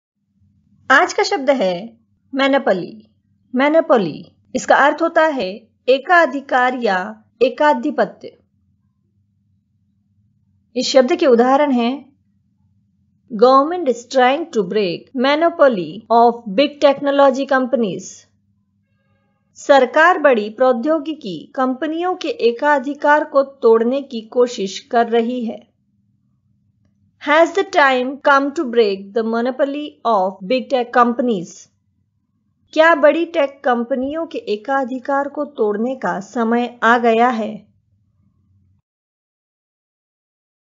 Monopoly meaning in Hindi
Monopoly  का हिंदी में मतलब.Monopoly  Meaning in Hindi with examples